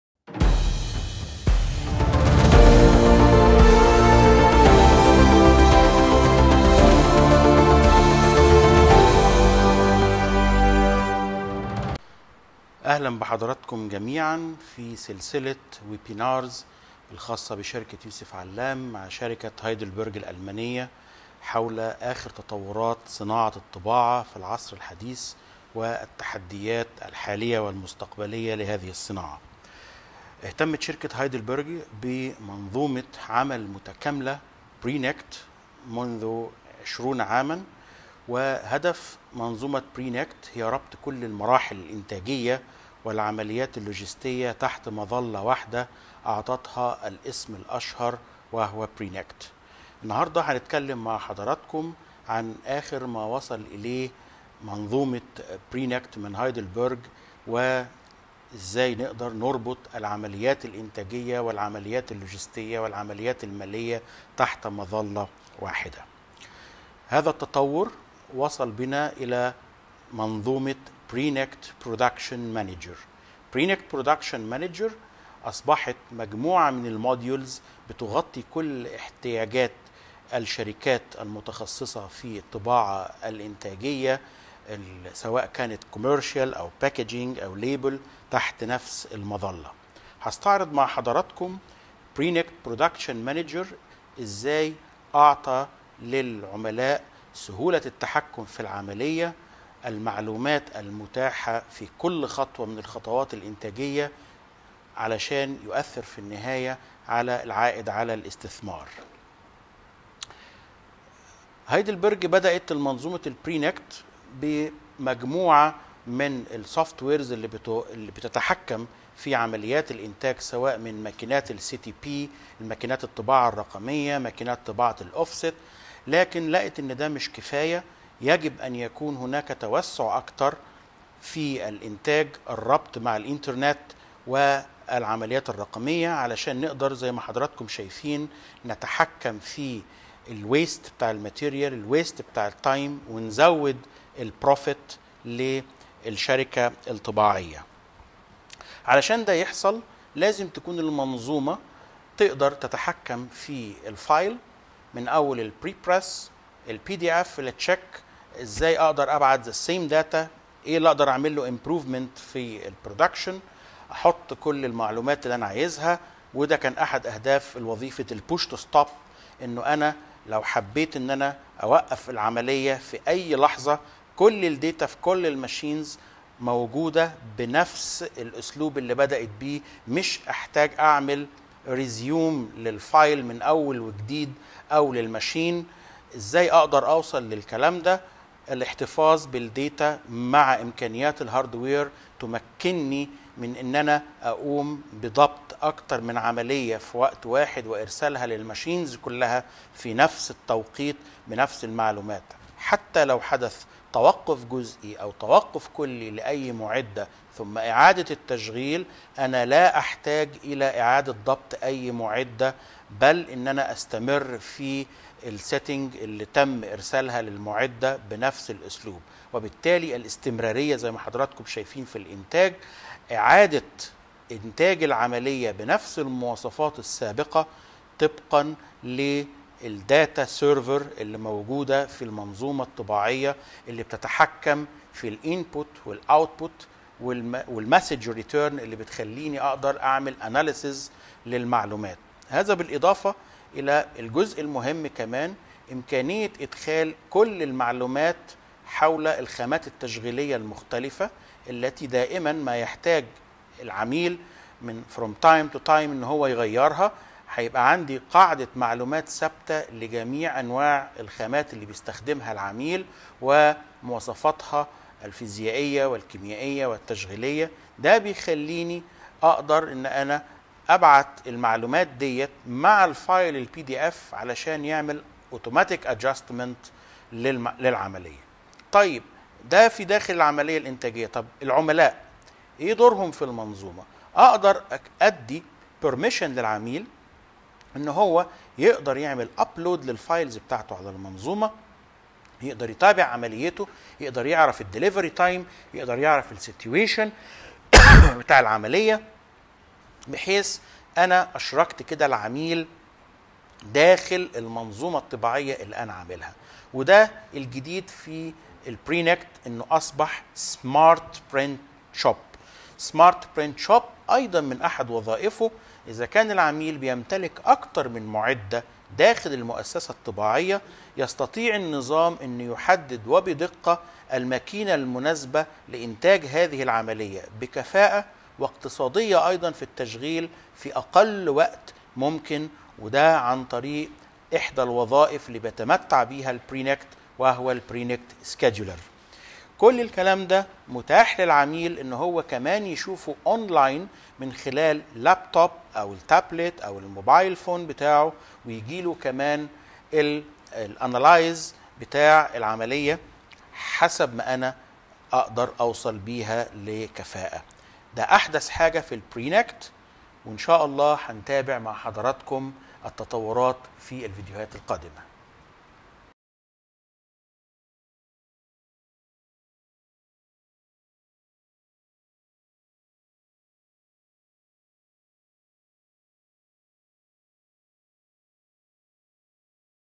Commercial Session in Arabic Part 2 - تحديات الطباعة التجارية الجزء الثاني
Commercial Session in Arabic- Part 2 / Eng. Ayman Taha
Heidelberg Innovation week
مهندس / أيمن طه
تحديات الطباعة التجارية – الجزء الثاني